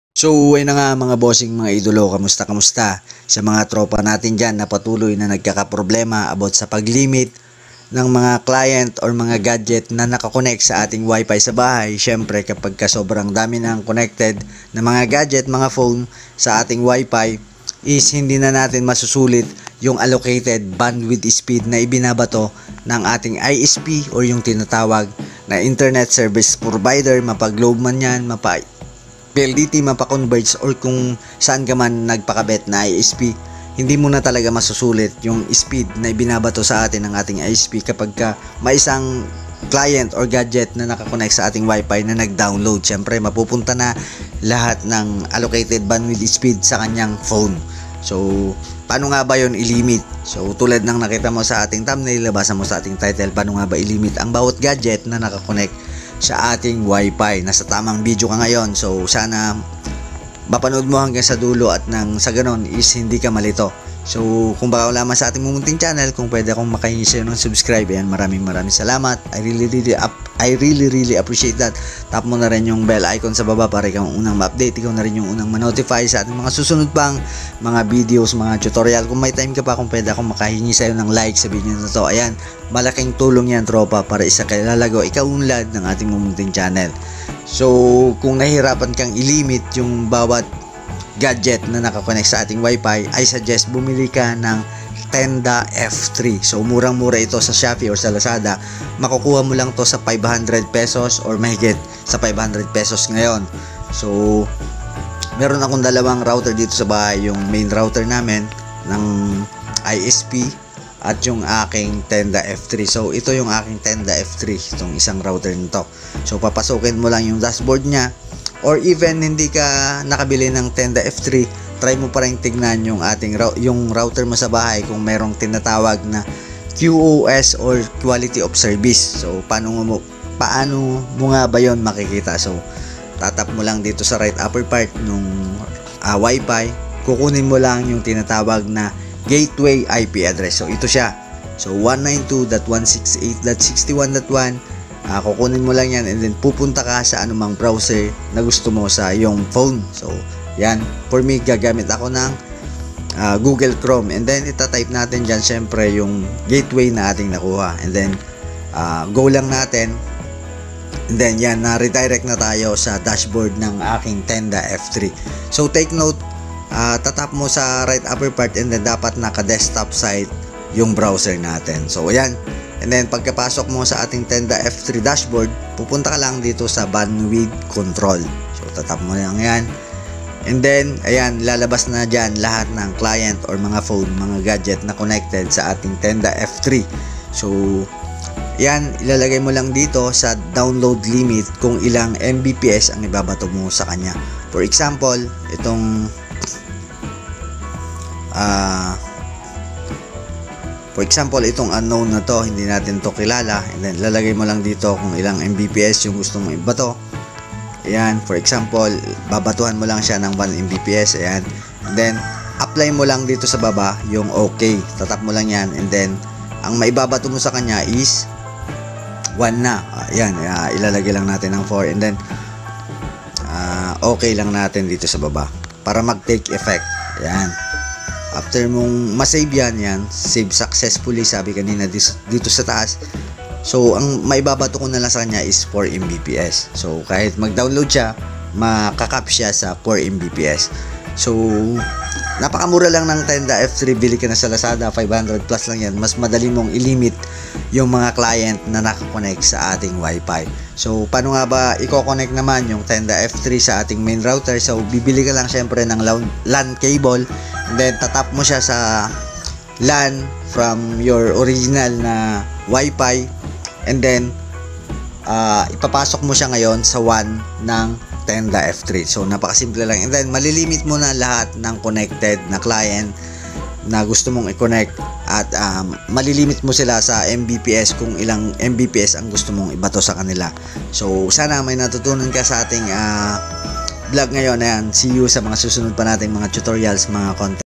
How to Limit Speed on WiFi users 2023
In this video, I will show you how to limit speed in every connected devices on your wifi. Please follow the steps carefully.

Kung gusto mo ang mga ganitong uri ng video, wag kakalimutang magsubscribe sa ating munting channel. Maraming salamat.